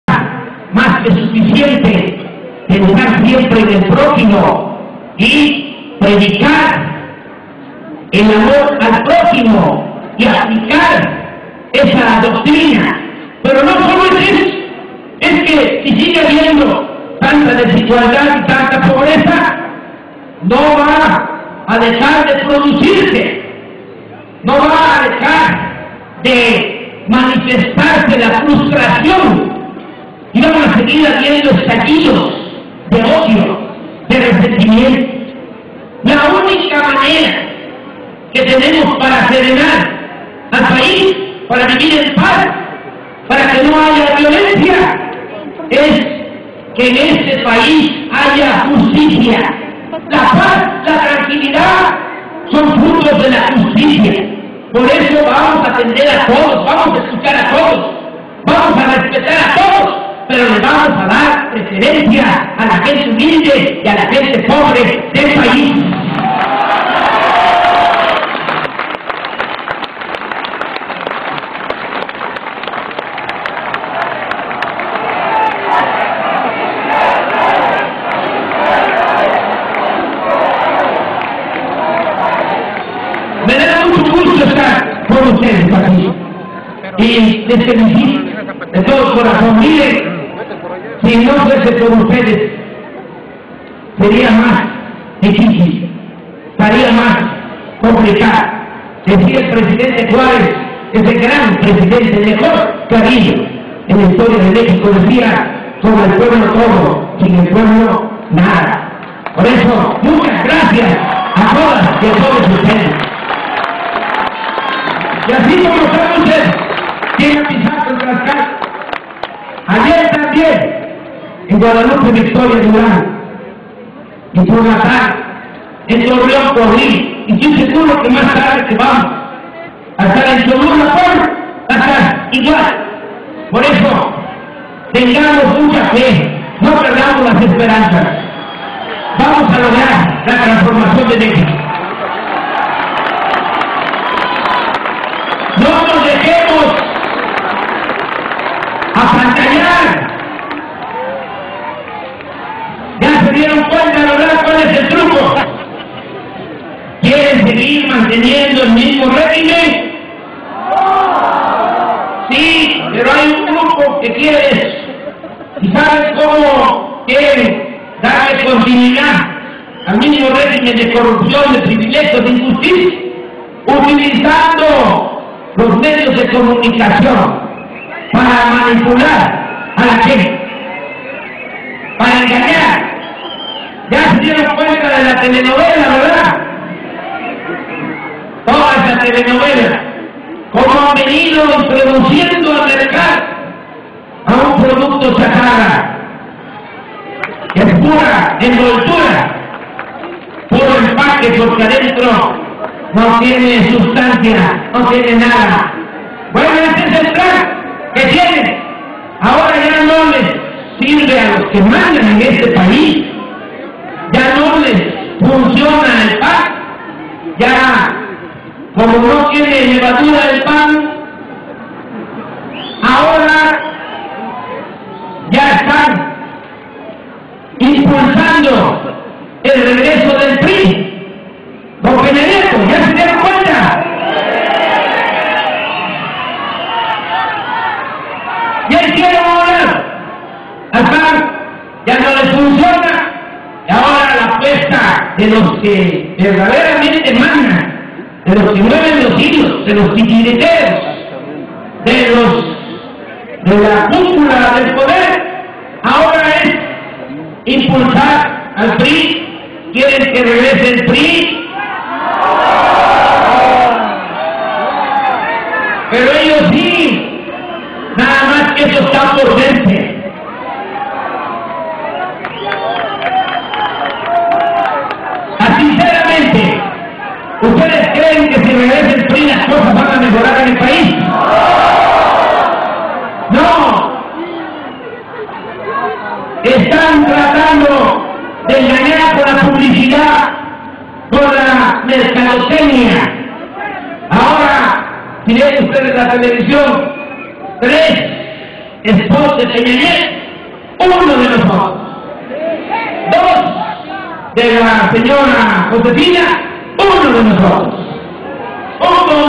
Qik - Se le acaba el amor a AMLO en Tlaxcala by Edgardo Cabrera
Streamed by Edgardo Cabrera in Apizaco, Mexico. Qik is the fastest way to upload videos to YouTube from your mobile phone.